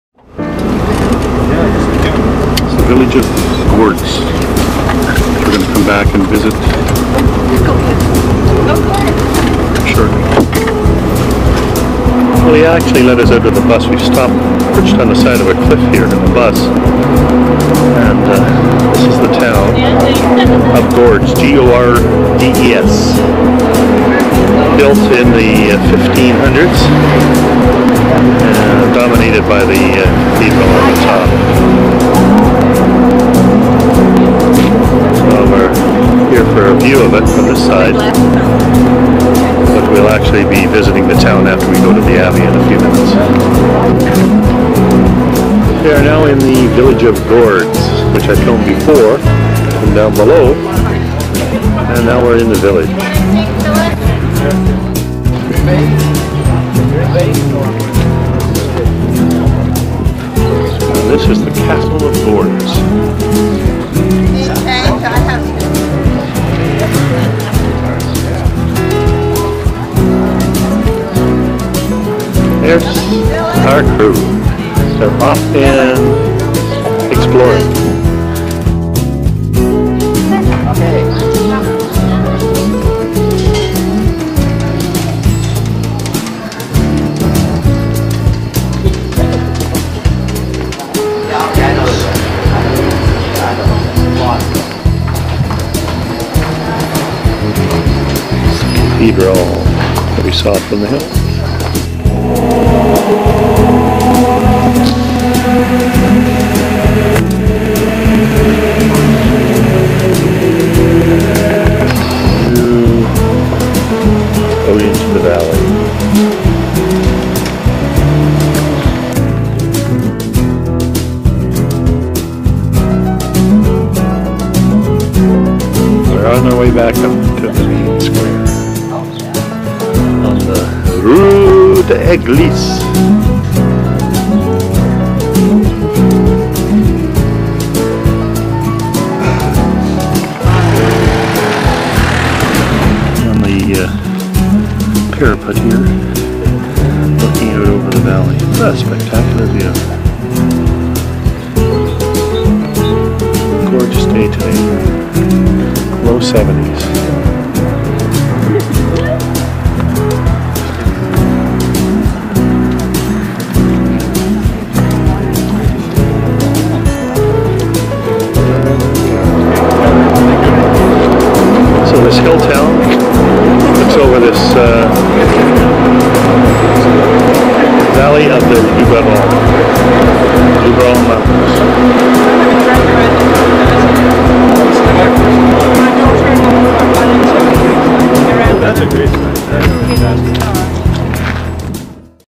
Village of Gordes, in the Luberon mountains of Southern France
Built on the foothills of the mountains of Vaucluse, facing the Luberon mountain range, the village of Gordes is one of the most well-known hilltop villages in the region and one of the most beautiful in France.
Its houses and buildings of white stone, root themselves into the sharp cliff of the mountain. Its labyrinth of narrow cobblestone streets do not leave the visitor indifferent to its charms.